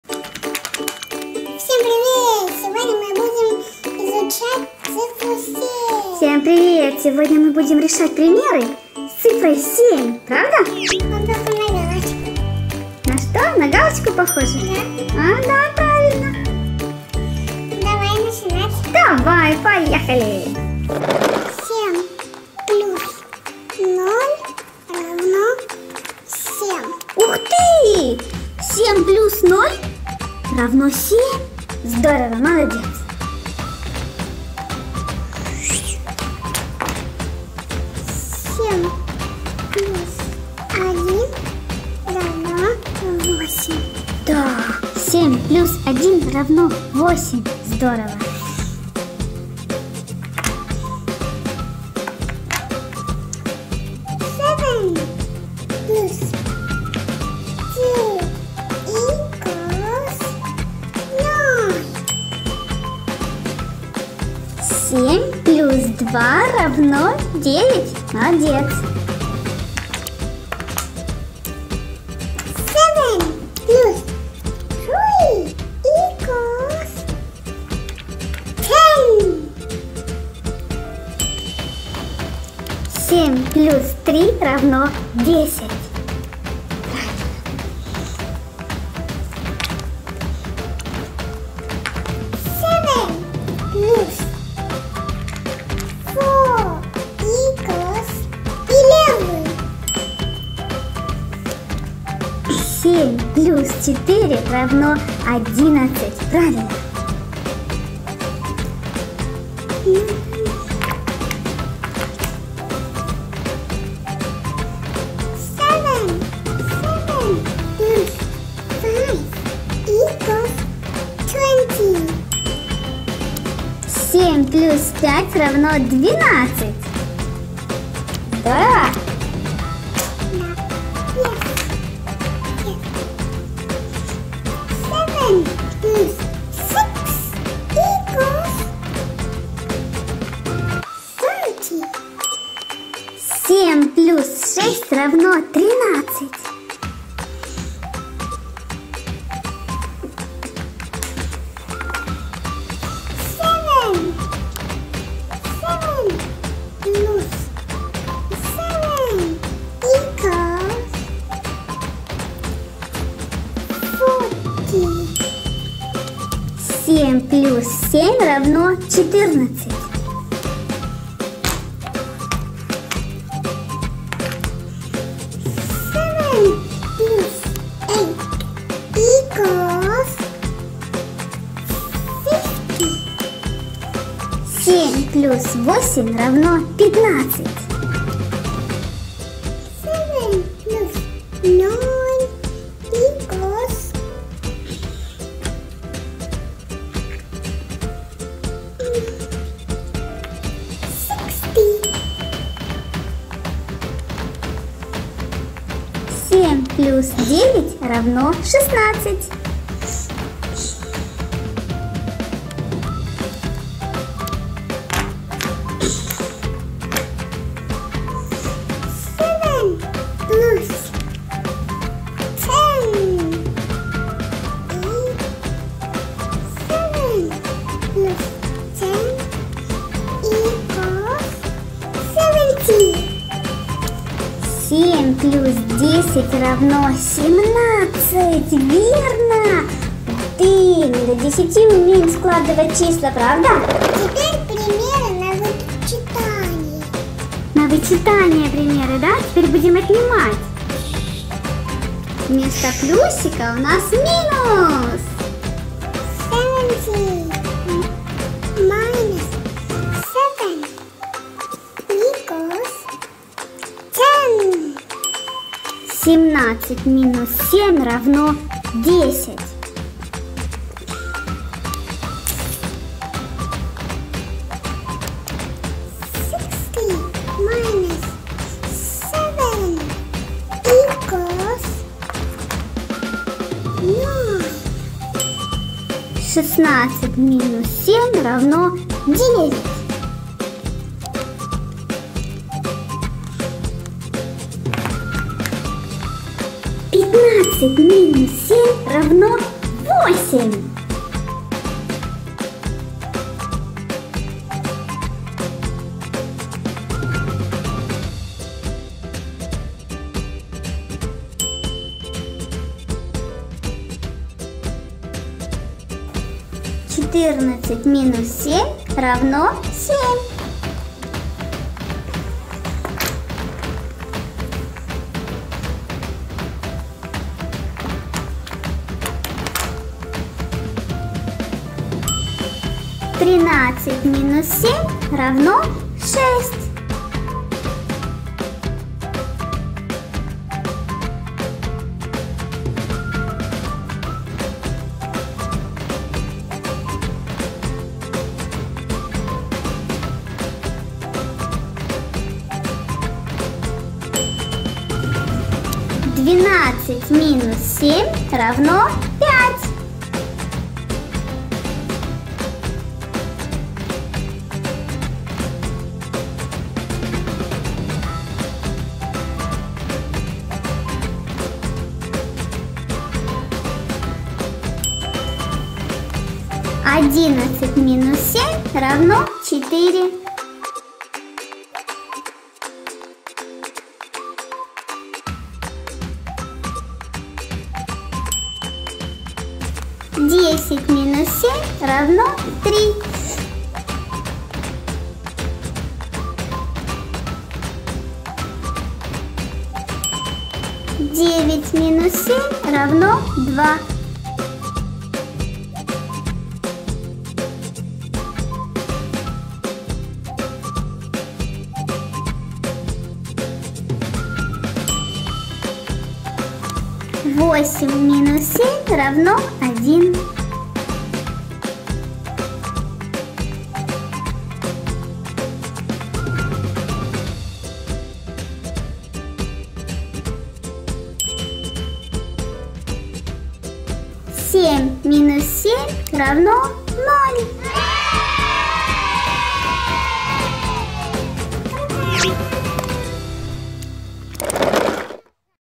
Цифра 7 Математика для малышей Примеры с цифрой 7 прибавляем и отнимаем
Всем привет! Вы на канале Egor&Co!!! Мы продолжаем нашу серию уроков по математике и сегодня у нас примеры с цифрой семь. Учим примеры на русском и английском языке.

Funny learning for children with Egor
Забавное обучение для детей с Егором